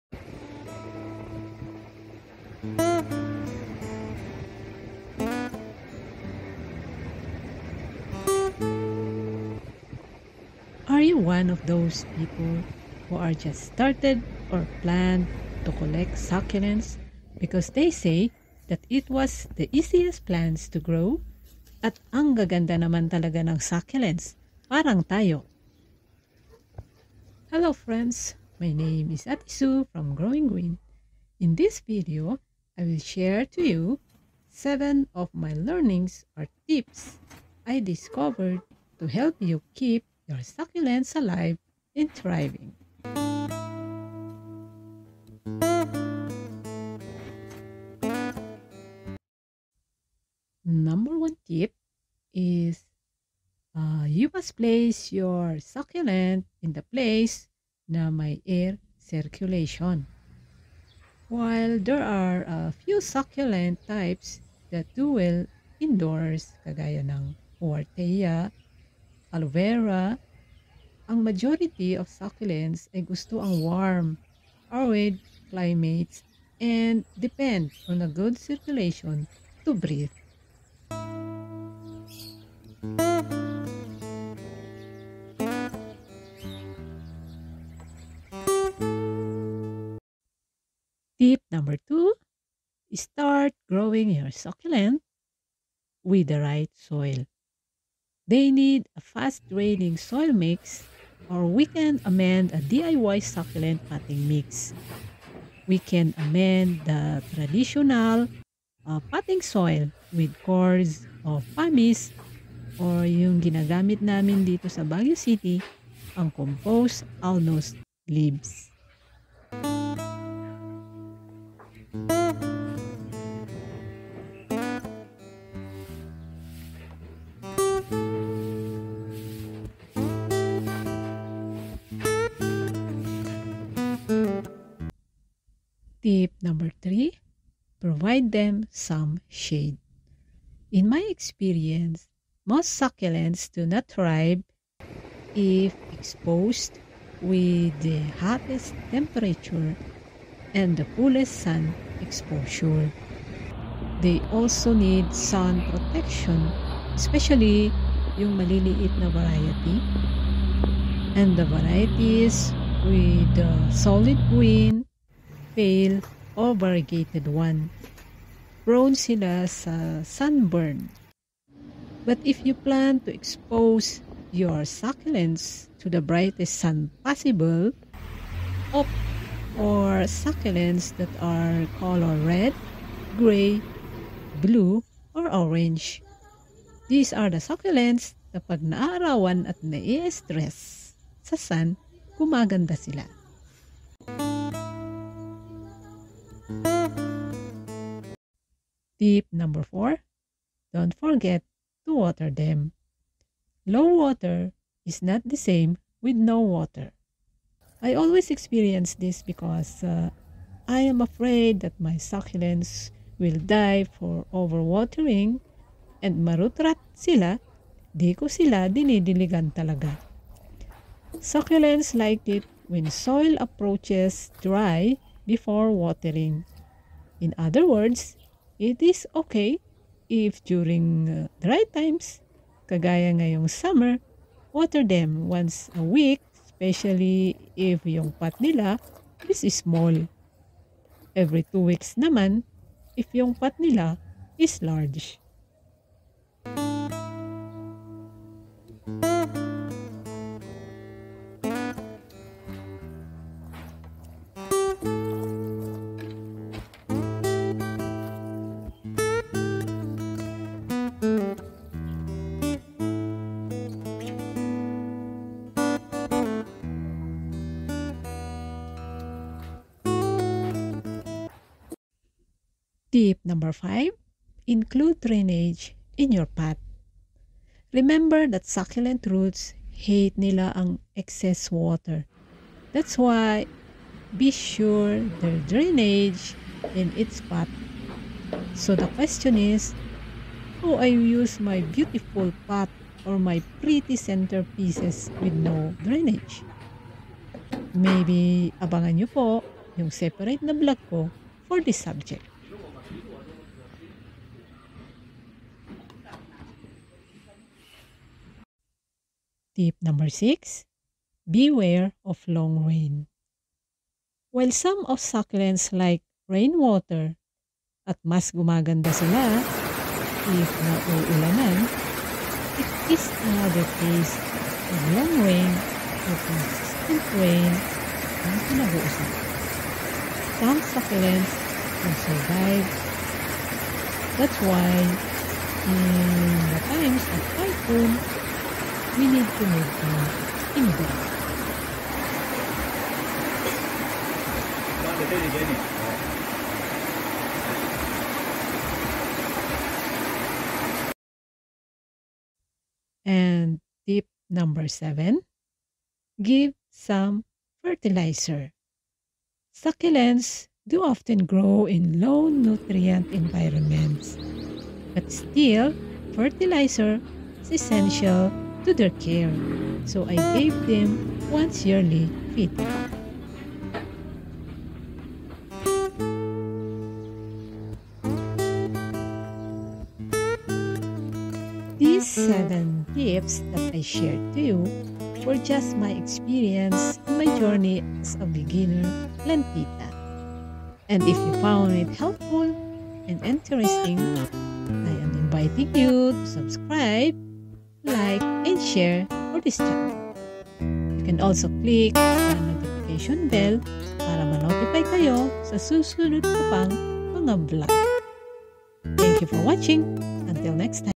Seven (7) ways to keep your succulents alive
This video features a beginner's experience in caring for succulents.
1. Air circulation
2. Start with the right soil
3. Provide them some shade
4. Don't forget to water them
5. Include drainage in your pot
6. Beware of the long rain
7. Fertilizer

Thanks,

Ate Sue